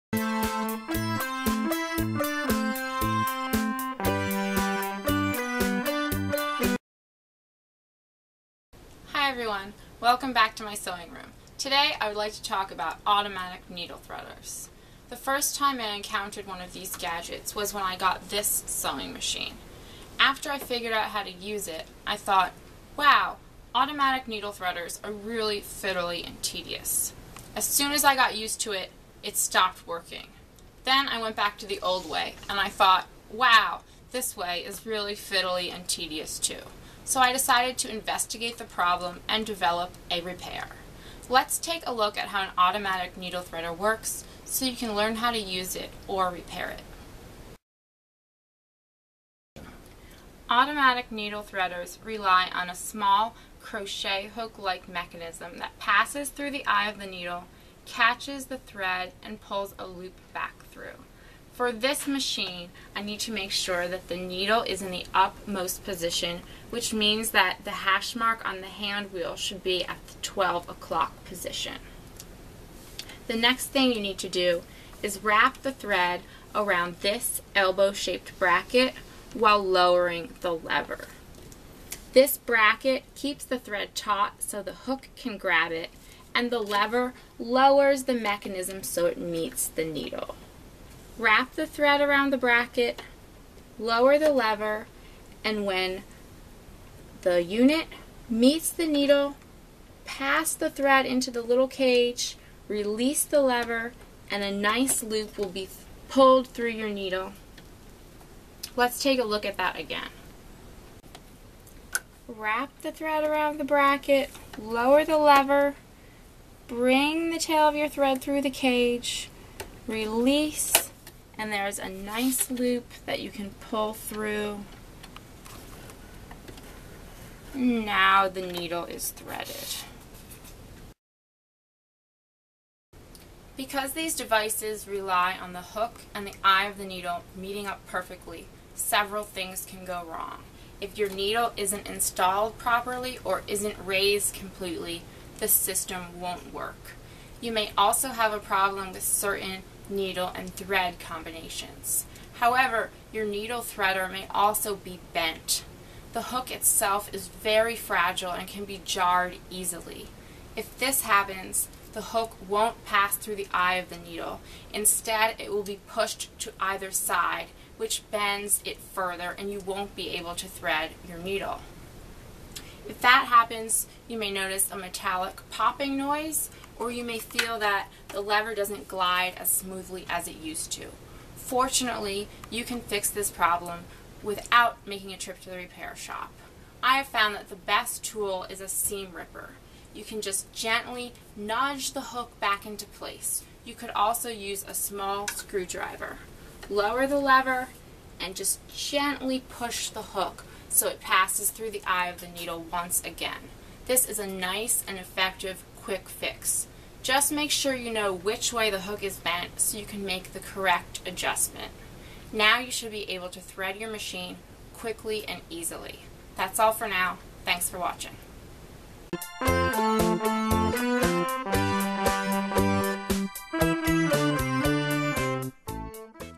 How to Use (or Fix) an Automatic Needle Threader
Don't let that automatic needle threader get you down! Learn how to use this funky gadget and fix it when it won't thread your sewing machine needle.

Check out the other videos in this series to learn more about the parts of your sewing machine and how they work.